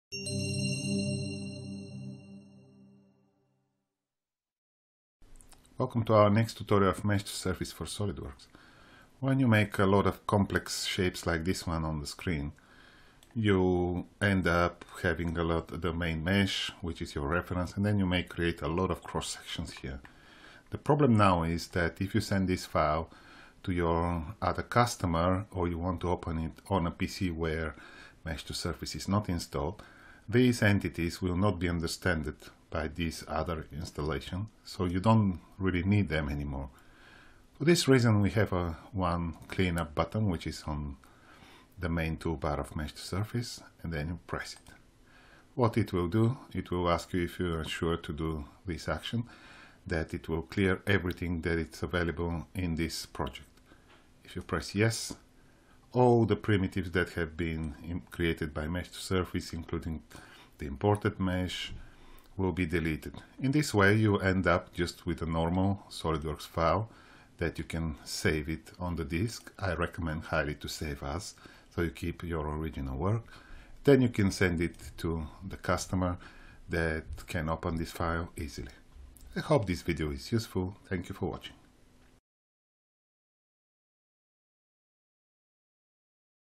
Scan To CAD - Mesh2Surface for SOLIDWORKS - Clean Up scan data
What do you need to do reverse engineering from scan data in SOLIDWORKS?
Mesh2Surface provides a set of efficient tools to generate surfaces and curves to help reconstructing a professional CAD models.
Easily load scan data in seconds, not minutes;
Extract quickly reference geometries;
Align intuitively in the right position in space;
Create reference sections for sketching;
Approximate free form surfaces with best fit surface;
Compare your work against the reference data instantly with 3D colour map;
Have a magic button to convert free form organic shapes just with the hit of a button;
And do all this in your favourite CAD package - SOLIDWORKS